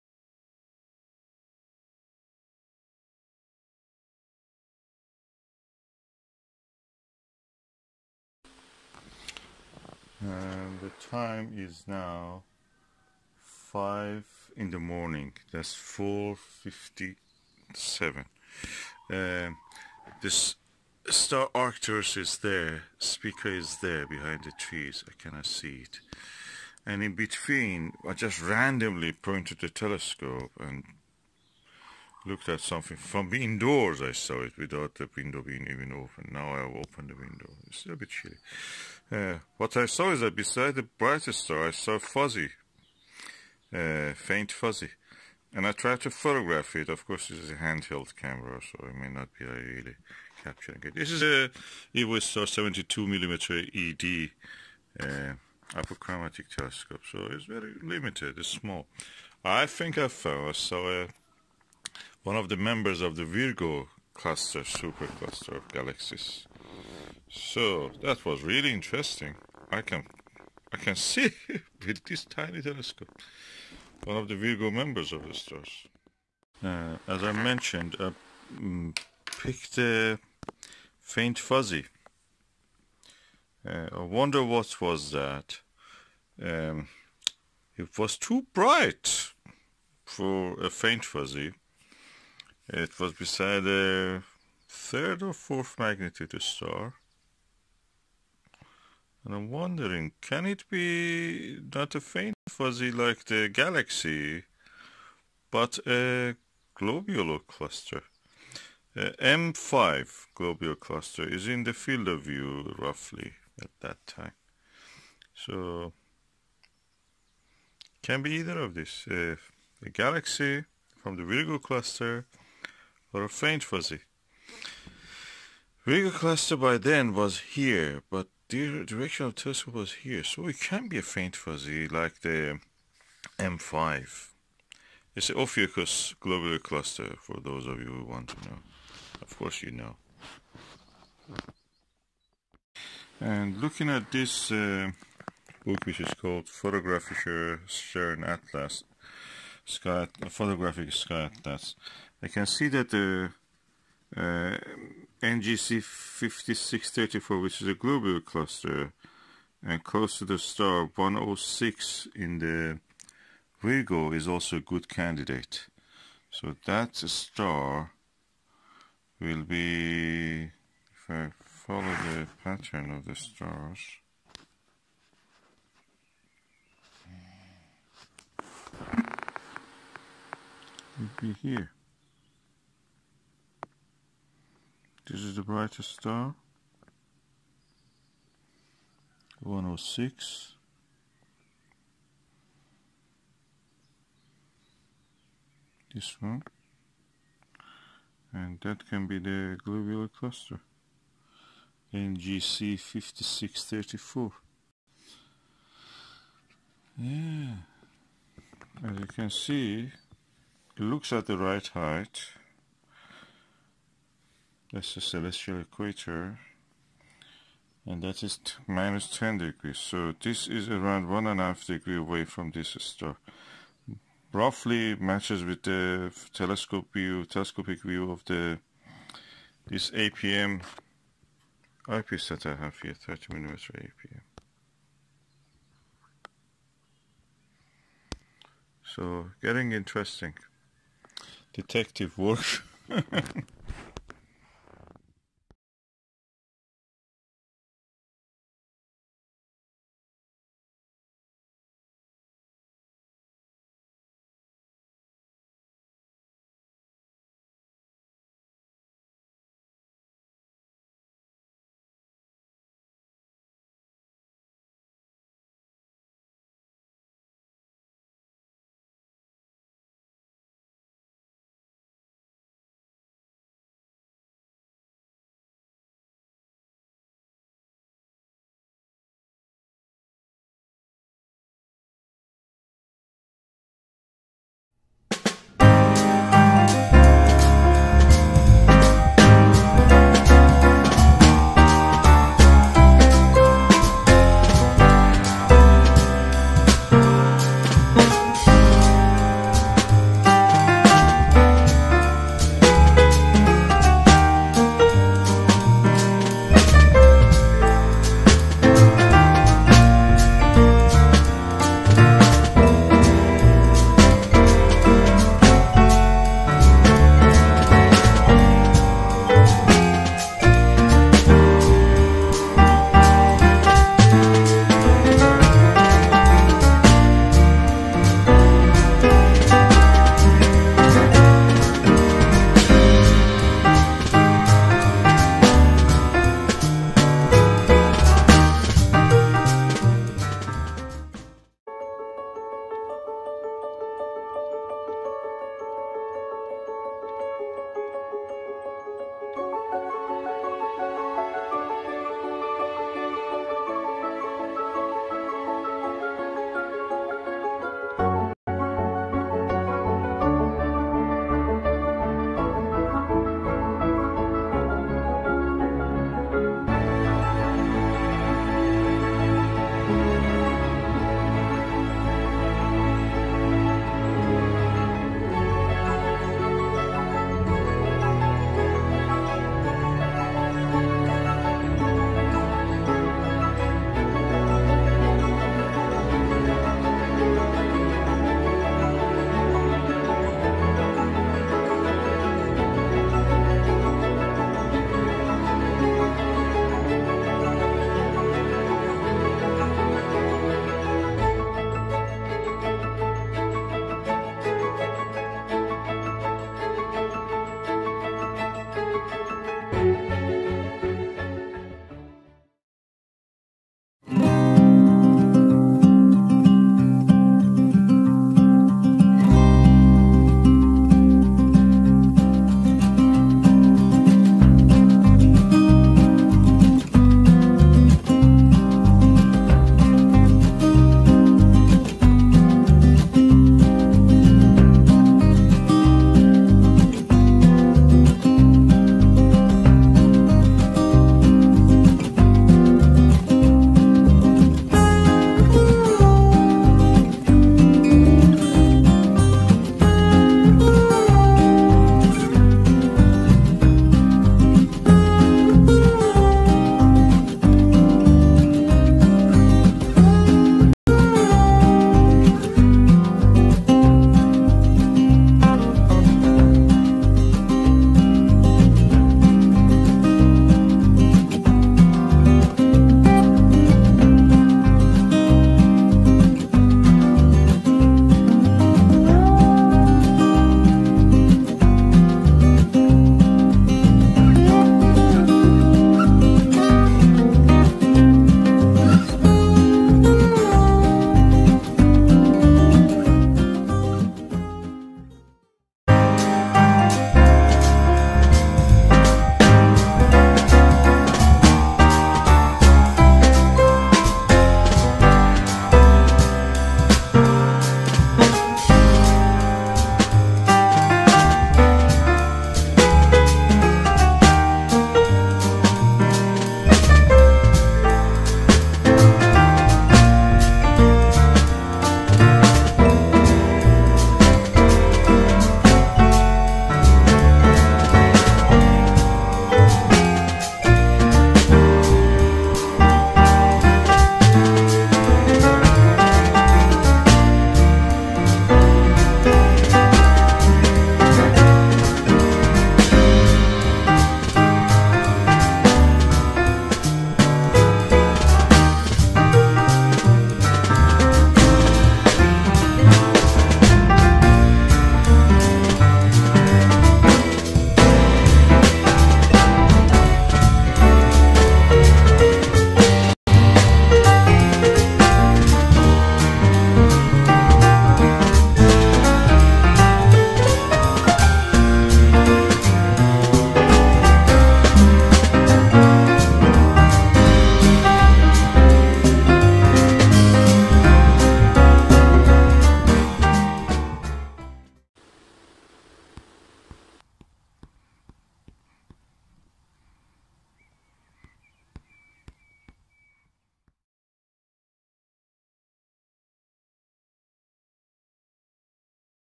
Observing Deep Sky Object With Skywatcher Evostar 72ED DS-Pro Apochromatic Refracting Telescope
I waked up early morning, around 4:57 todayin self isolation due to coronavirus lock down. It was frost outside but crystal clear. Without opening the window I set up my Skywatcher Evostar 72ED with my new APM HDC 13mm 100 Degree Eyepiece and started to sweep the sky for interesting objects. I came across this magnitude 4 star and noticed a Fuzzy patch to 11 o'clock of it. I thought the eyepiece is out of focus, but no, it was there in real. I think I may have observed either the M5 globular cluster in  Serpens or a galaxy in Virgo. I opened the window and could see it more clear. It was Fuzzy at centre but a tiny faint halo was around it. The star did not display this halo.